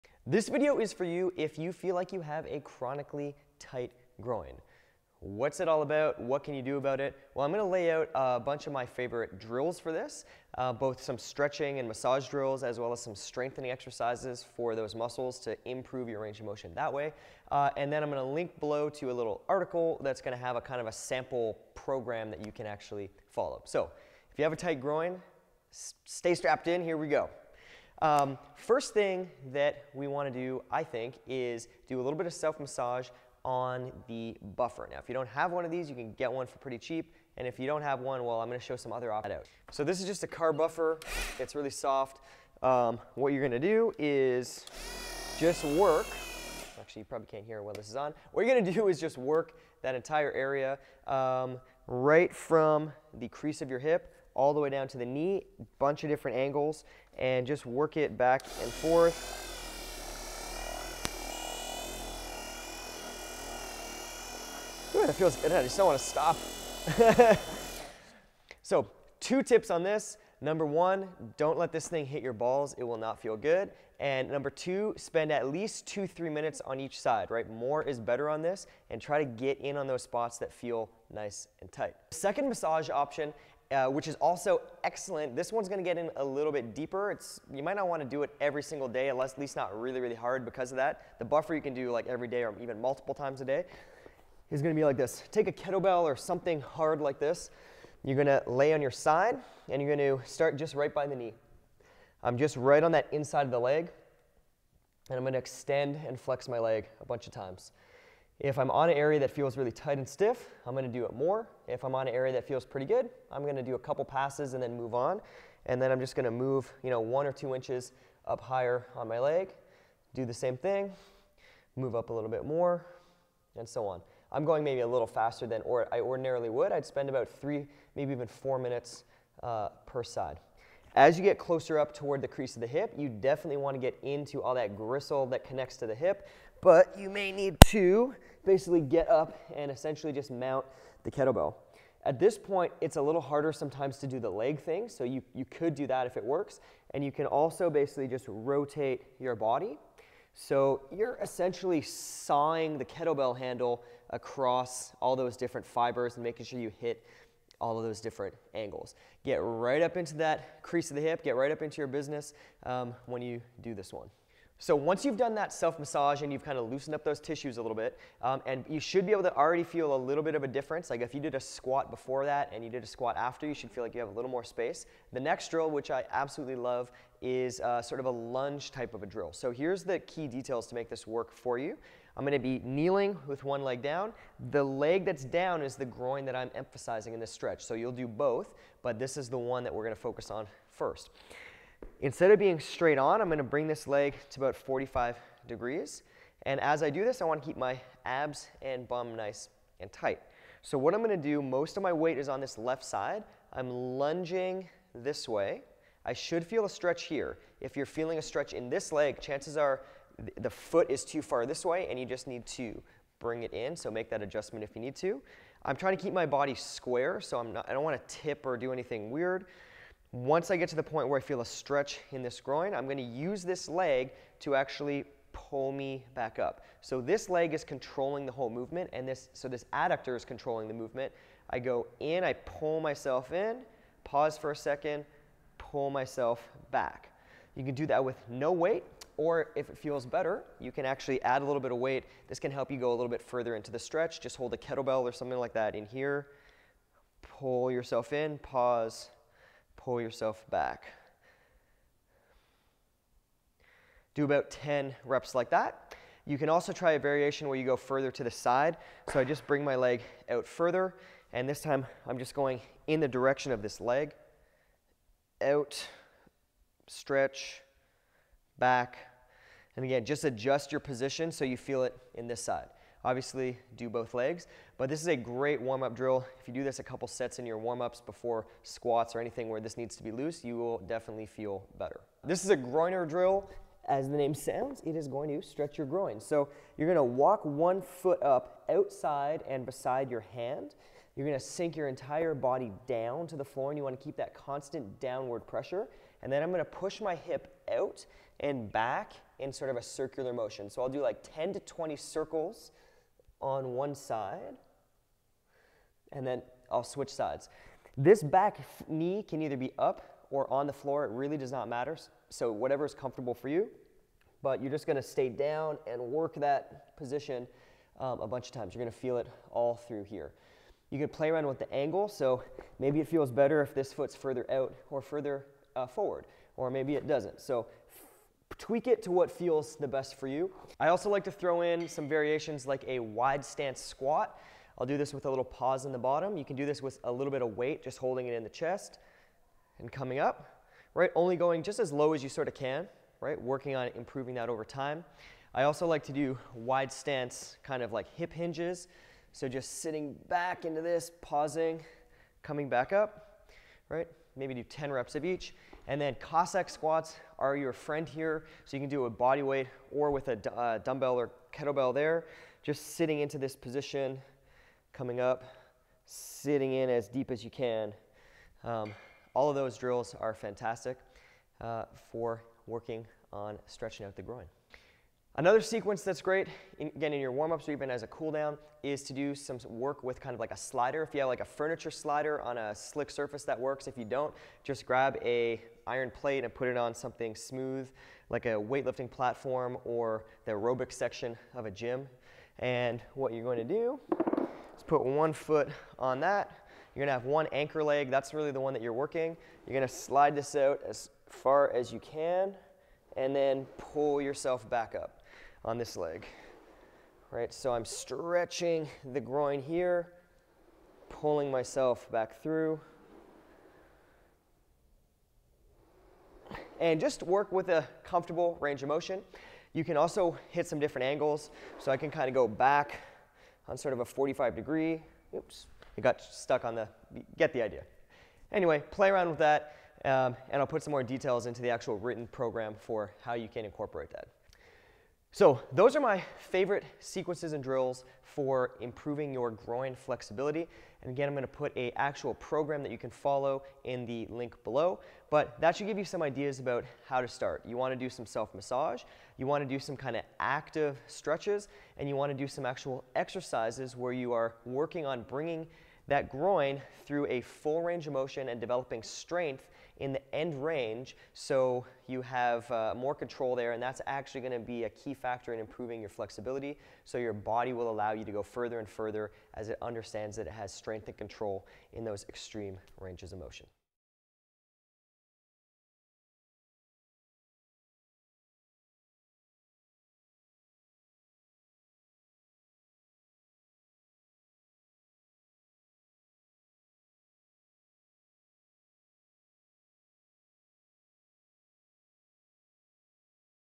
What to do if you have a chronically tight groin
What to do for  a chronically tight groin?

This is for you if you have a tight groin that just doesn’t want to budge. Tight adductors (i.e. your groin muscles) can cause issue with knee pain, hip/SI pain, and can generally restrict your movement in squats, running and skating (for the hockey players out there).

Here are my favourite techniques to get you started on solving a tight groin. We’re going to use a combination of outstanding massage, stretching and strengthening drills to attack the problem. Generally speaking this isn’t just a flexibility issue. It’s also a strength issue. Weak adductors are tight adductors.

SAMPLE PROGRAM

Here’s a place to start if you’re not sure how to order the exercises.

The key is consistency. With any long-standing mobility limitation don’t expect results if you aren’t working on it regularly. That means daily. Ideally multiple times a day. Worst case every other day. In other words, if you want to see real change you need to make it a priority.

SIMPLE 10-MINUTE DAILY ROUTINE
-Buffer Self Massage x 2mins/side
*Every 3-5 days you can try the deeper groin massage with the kettlebell handle.

-Groiner Circles x 2mins per side

-Cossack Squats x2mins. Perform with a 5sec pause in the bottom position each rep. Alternate sides.
*As you get better at this, add load with a kettlebell or dumbbell.
**If Cossack squats are too difficult at first, do wide stance squats and wide stance hip hinges instead. Start with bodyweight. As you advance, add weight gradually by holding a dumbbell or kettlebell in your chest, similar to a goblet squat.

PRE-ACTIVITY PREP

-3 sets, alternating exercises, done with slow/controlled tempo:-Kneeling Groin Stretch x 45sec/side (with weight if able)-Groiner Circles x45sec/side

-then:
3 sets, alternating exercises, with controlled tempo and moderate weight:-wide stance RDL x 10 reps-Slider Drills x 5-10/side OR cossack squats x 10/side
*If you’re proficient with an exercise called a Windmill, that can work well too.